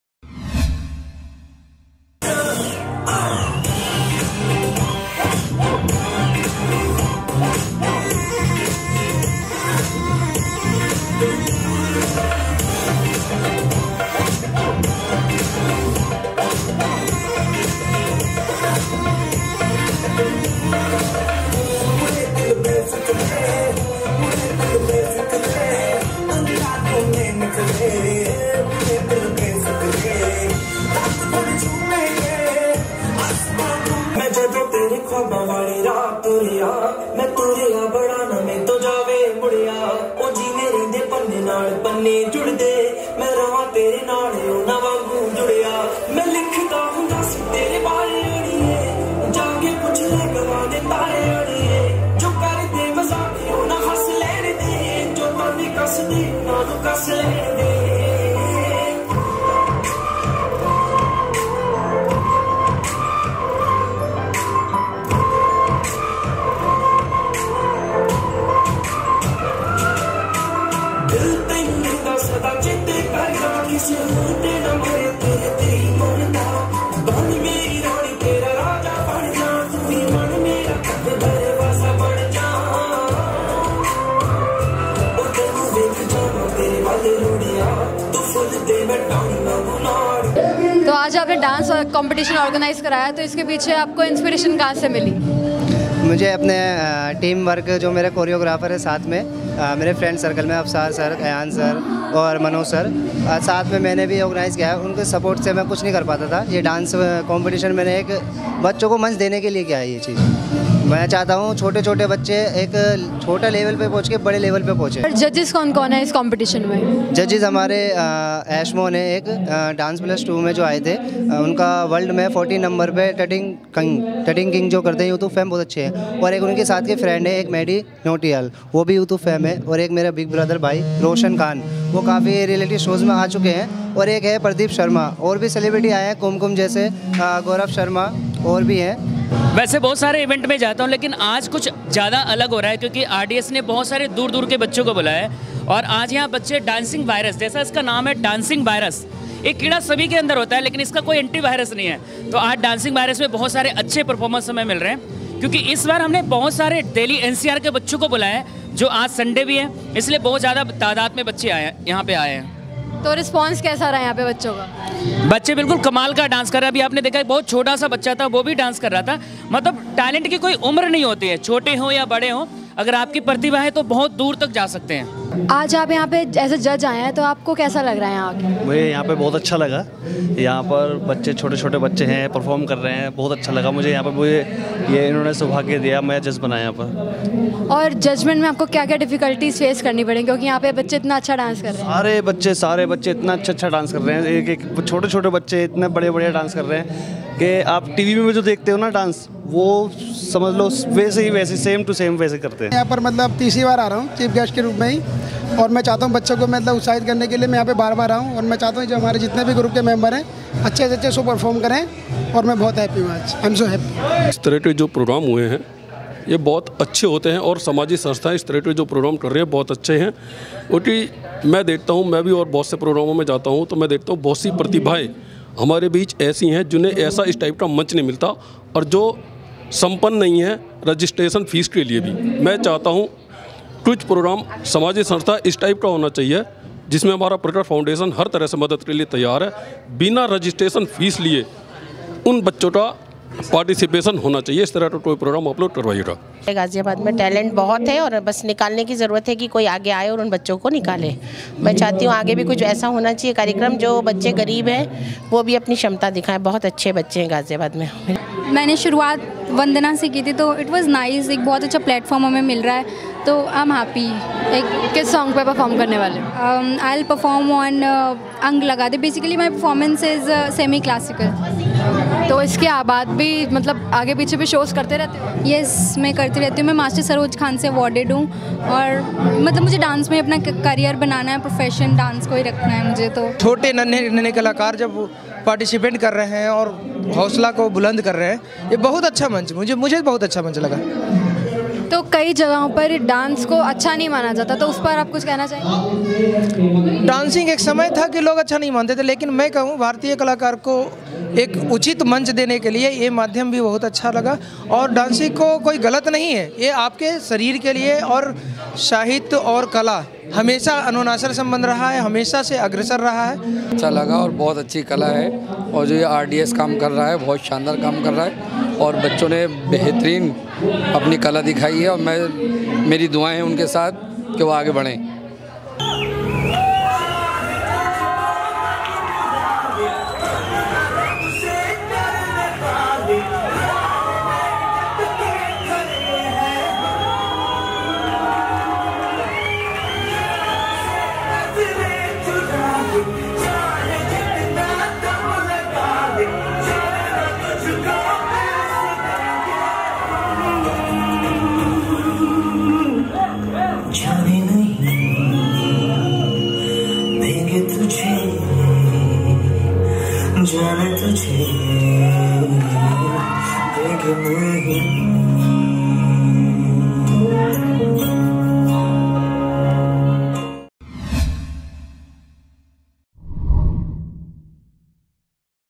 "DANCING VIRUS" Dance Competition Organized at Ghariabad | Face News | 2017
Group: Face Group
Channel: FACE NEWS
Anchour : Loveena Bahl
Camera Team: Bilal Ansari